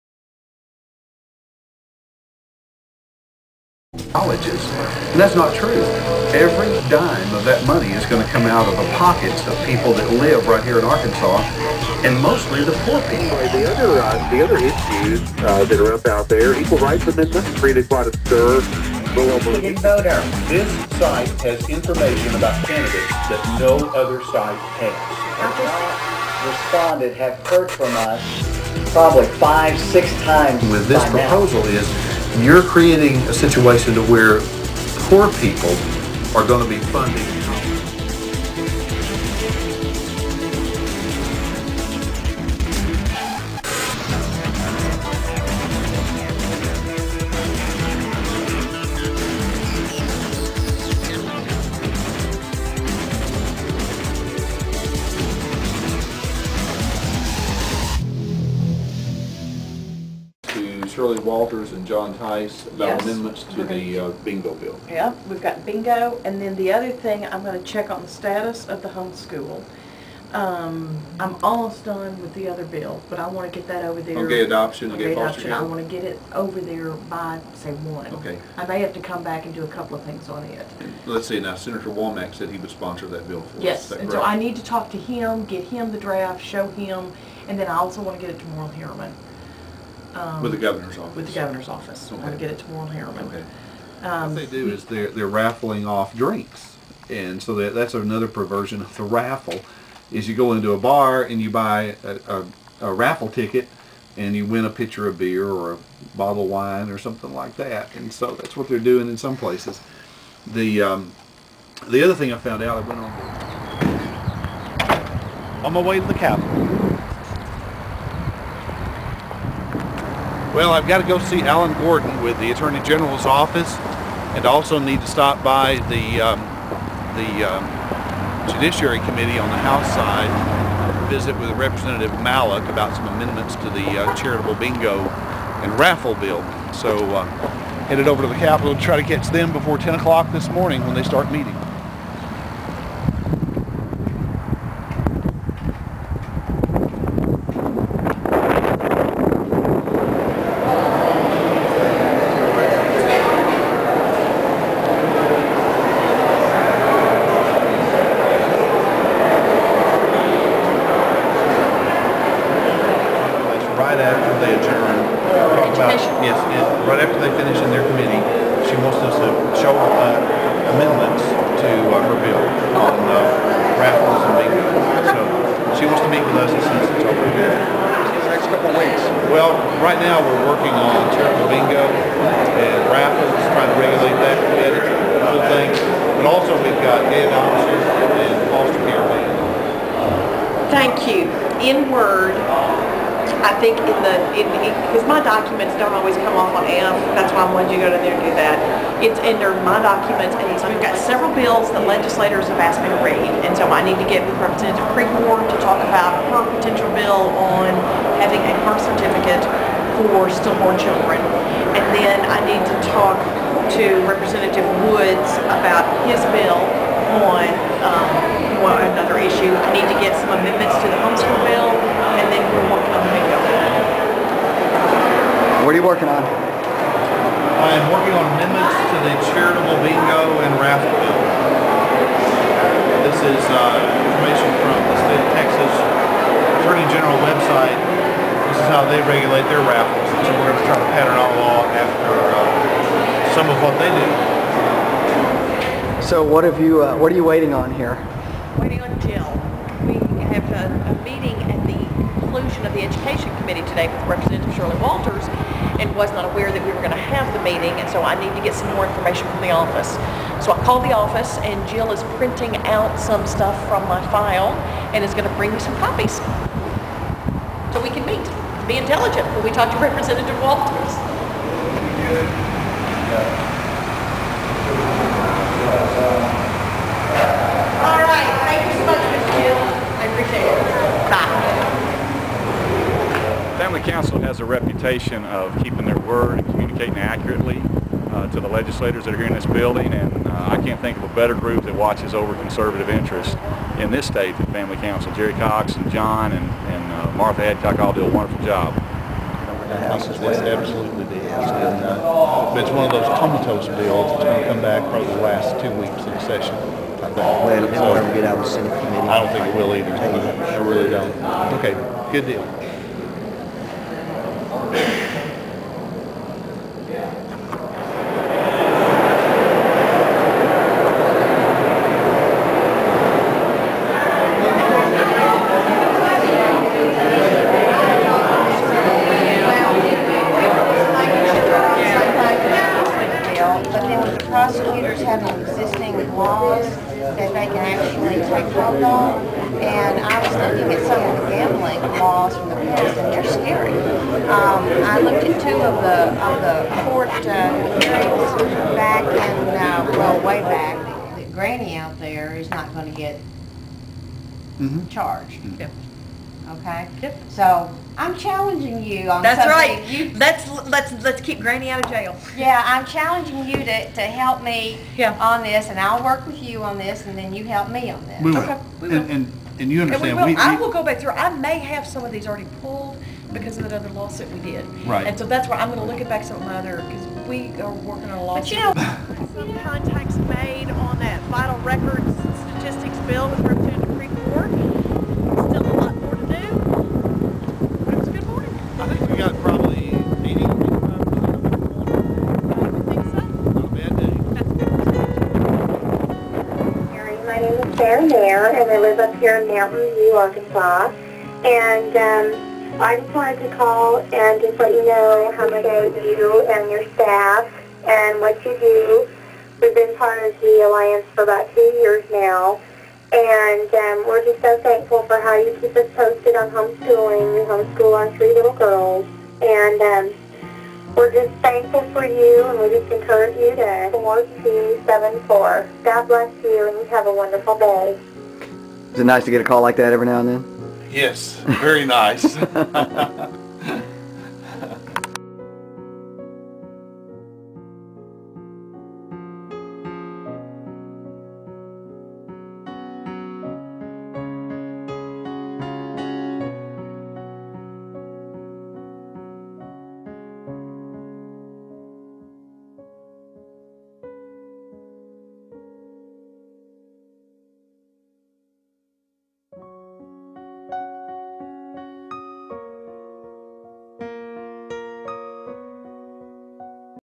A Day in the Life of Arkansas Family Council
A short documentary of a day lobbying the Arkansas State Legislature.